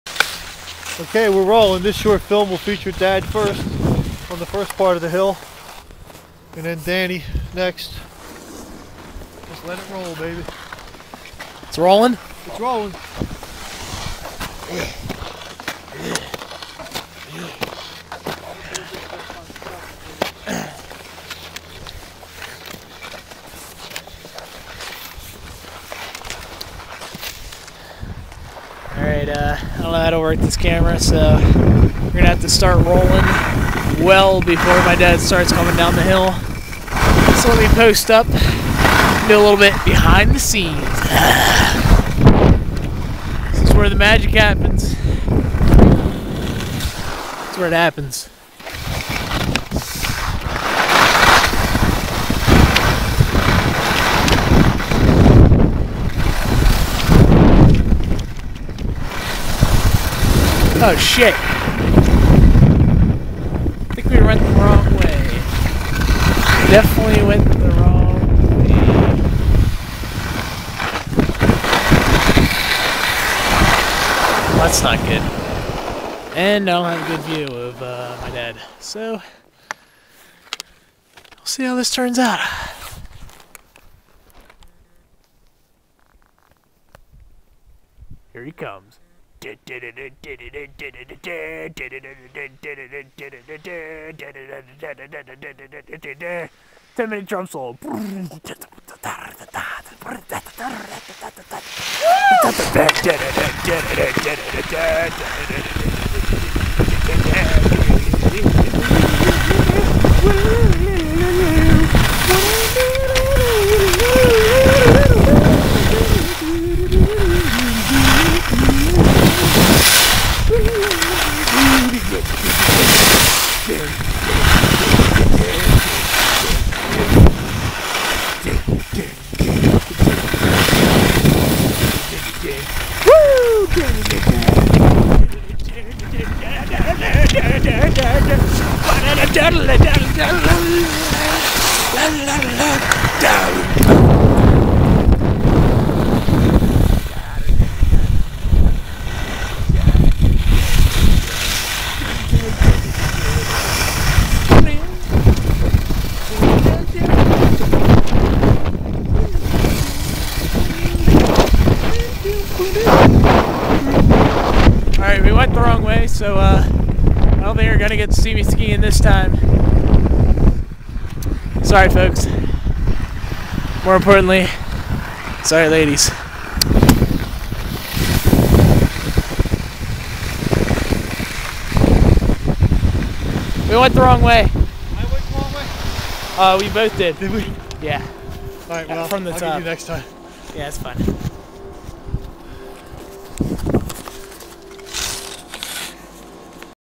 Hunter Mountain Danny skiing: Funny narration and sounds effects of ski run
Hunter Mountain skiing: Funny narration and sounds effects of ski run